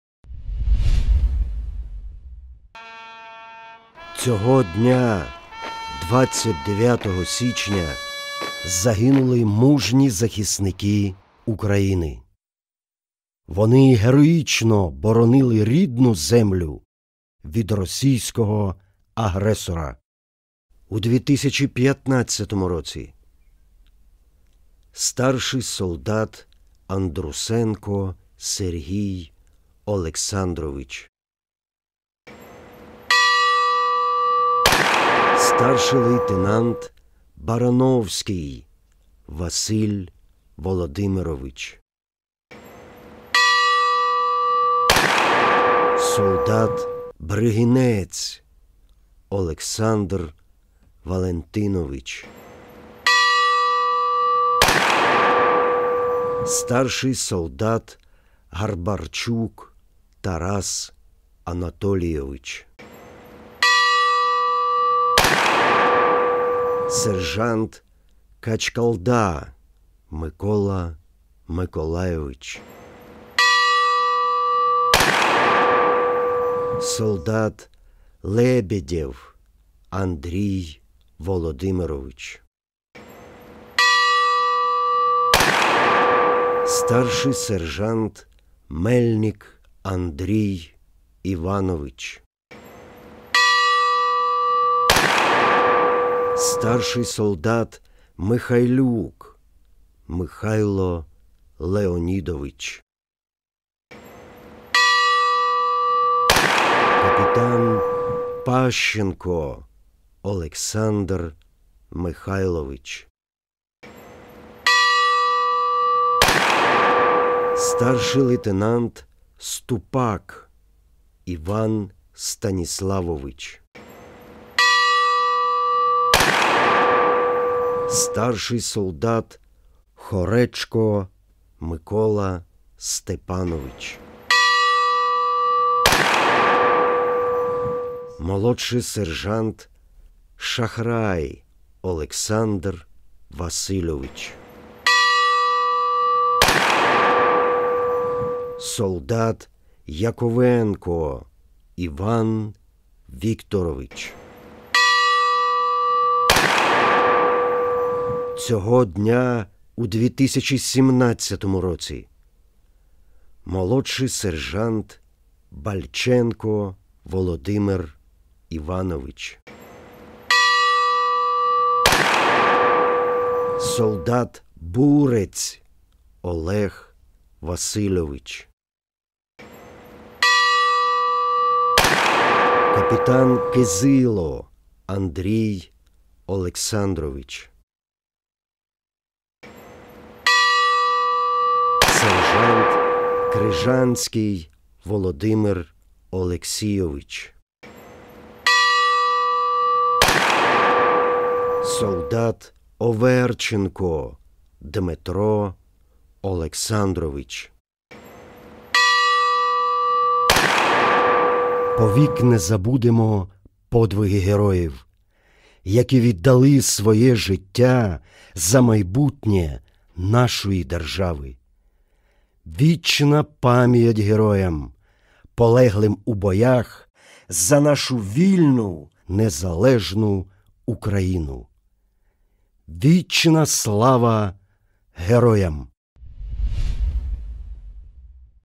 Ранковий церемоніал вшанування загиблих українських героїв 29 січня
Дзвін Пам’яті пролунав вісімнадцять разів…
У цей день внаслідок бойових дій  з російськими окупантами у 2015 році загинули:
Старший солдат АНДРУСЕНКО Сергій Олександрович
Старший лейтенант БАРАНОВСЬКИЙ Василь Володимирович
Солдат БРИГИНЕЦЬ Олександр Валентинович
Старший солдат ГАРБАРЧУК Тарас Анатолійович
Сержант КАЧКАЛДА Микола Миколайович
Солдат ЛЕБЄДЄВ Андрій Володимирович
Старший сержант МЕЛЬНИК Андрій Іванович
Старший солдат МИХАЙЛЮК Михайло Леонідович
Капітан ПАЩЕНКО Олександр Михайлович
Старший лейтенант СТУПАК Іван Станіславович
Старший солдат ХОРЕЧКО Микола Степанович
Молодший сержант ШАХРАЙ Олександр Васильович
Солдат ЯКОВЕНКО Іван Вікторович

У цей день у 2017 році загинули :

Молодший сержант БАЛЬЧЕНКО Володимир Іванович
Солдат БУРЕЦЬ Олег Васильович
Капітан КИЗИЛО Андрій Олександрович